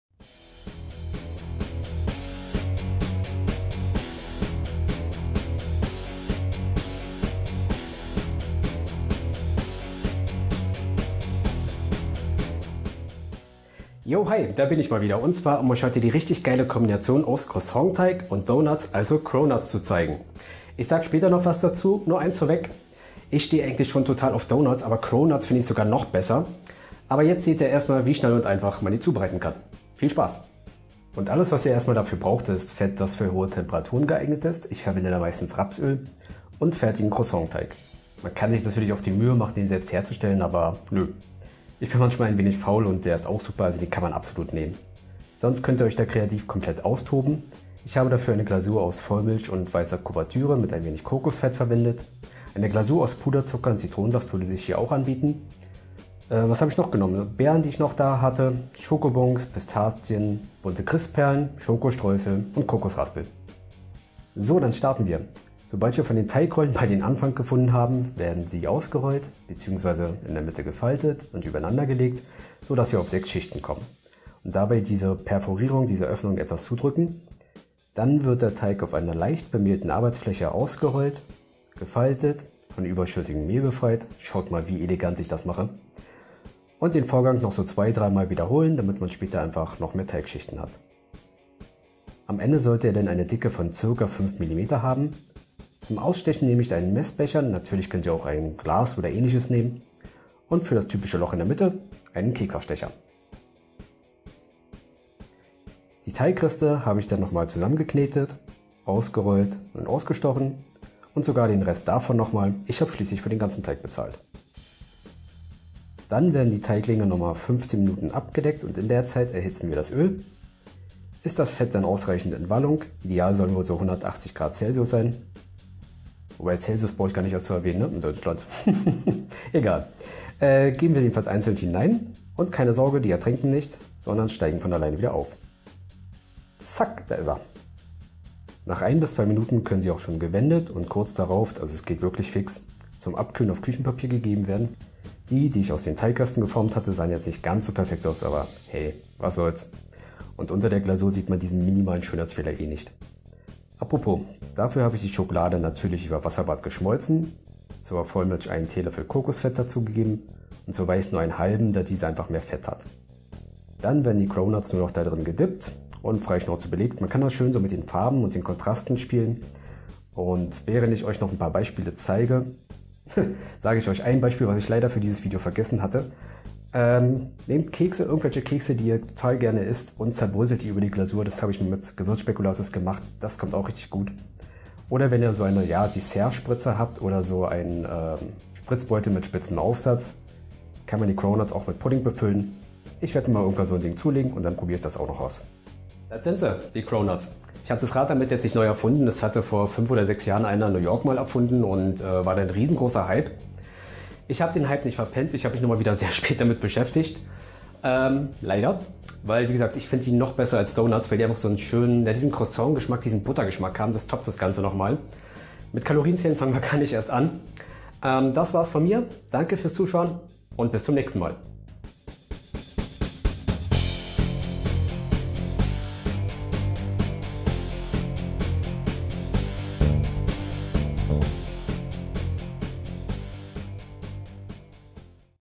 CRONUTS | Rezept | Crossiant Donuts
Ihr liebt Donuts auch? Dann müsst Ihr unbedingt mal Cronuts ausprobieren!

Zutaten für ca. 12-15 Cronuts
- 3 Rollen fertiger Croissant-Teig
- min. 1 L Fett zum Frittieren
- Rest  nach Wahl (Kuvertüre, Beeren usw.)